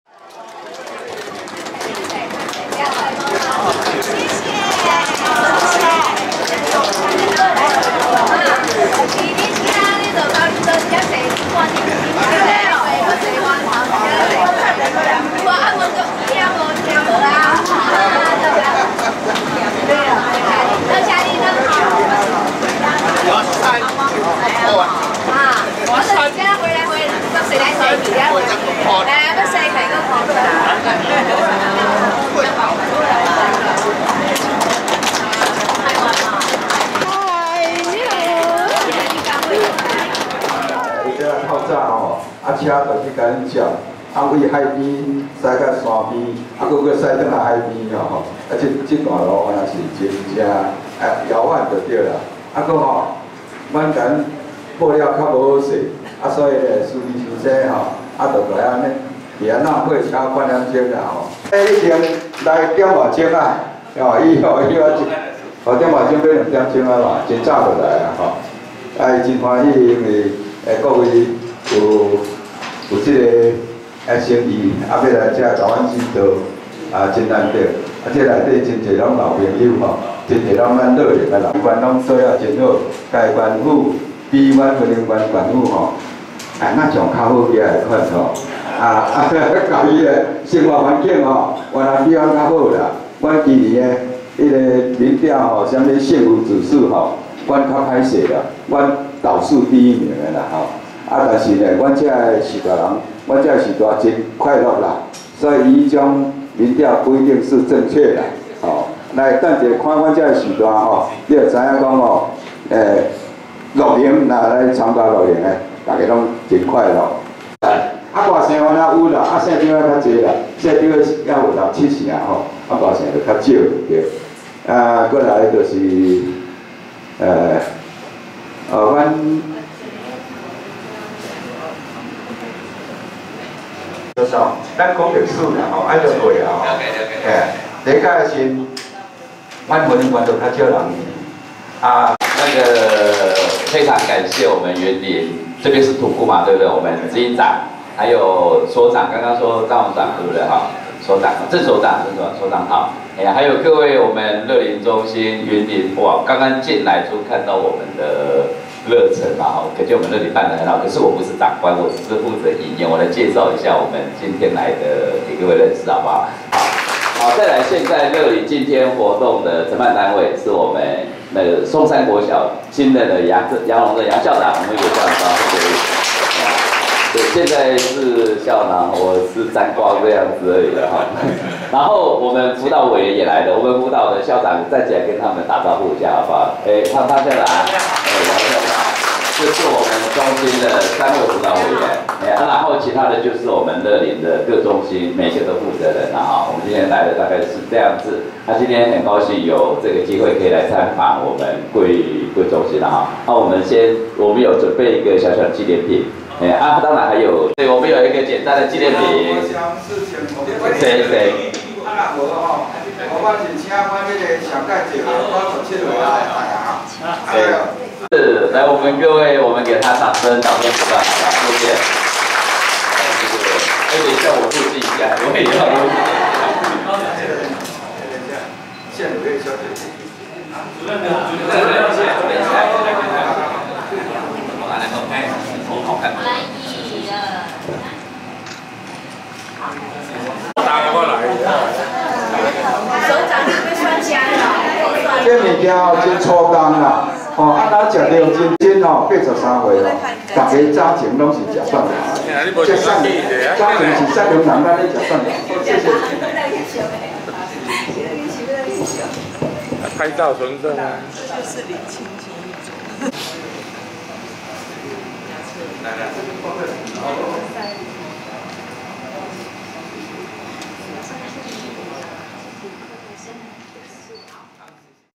嘉義縣樂齡工作夥伴來訪- 相見歡並互贈禮物1-雲林縣土庫鎮樂齡學習中心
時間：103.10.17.
地點：雲林縣土庫鎮樂齡學習中心